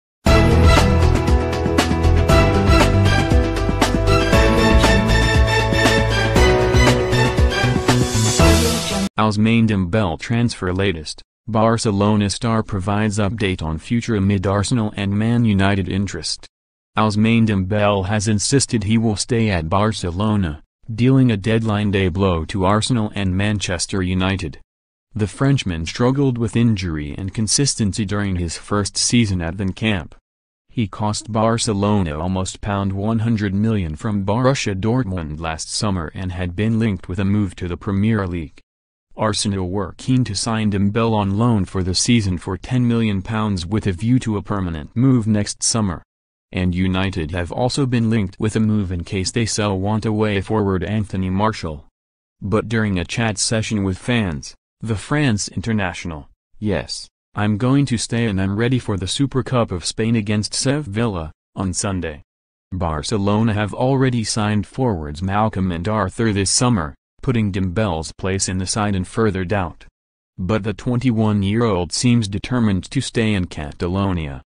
Ousmane Dembele transfer latest Barcelona star provides update on future amid Arsenal and Man
Ousmane Dembele transfer latest: Barcelona star provides update on future amid Arsenal and Man United interest. Ousmane Dembele has insisted he will stay at Barcelona - dealing a deadline day blow to Arsenal and Manchester United.

The Frenchman struggled with injury and consistency during his first season at the Nou Camp.

He cost Barcelona almost £100million from Borussia Dortmund last summer and had been linked with a move to the Premier League.

Arsenal were keen to sign Dembele